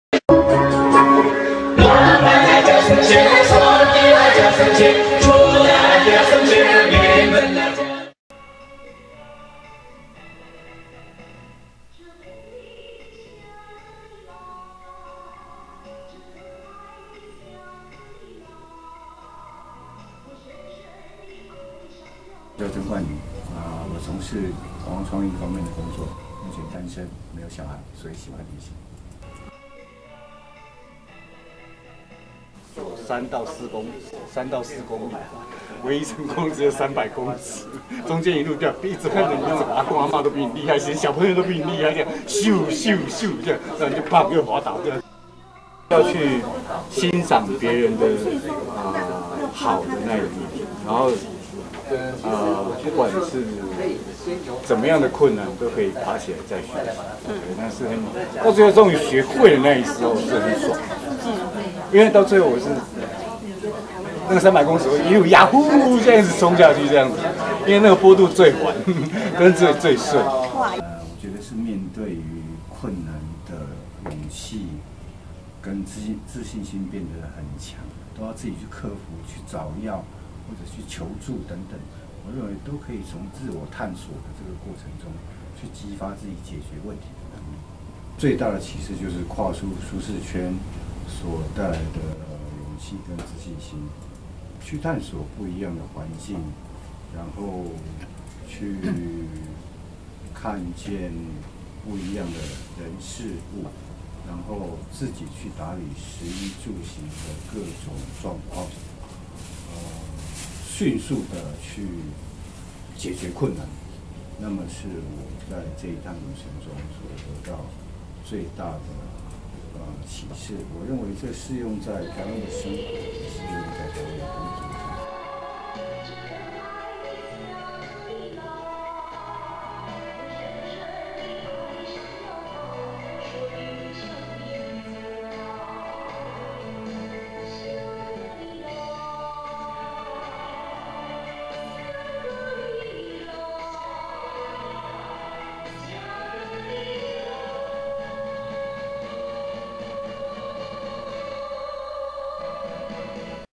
272-跨出舒適圈/香格里拉遊者訪談/ARIA製作剪輯配樂
跨出舒適圈/香格里拉遊者訪談/By Aria - 後製 剪輯 配樂

影音課程/社群經理人養成班--作業第一篇
小組共9人，錄製好訪談影片，再由個人剪輯製作完成。

8K108K0831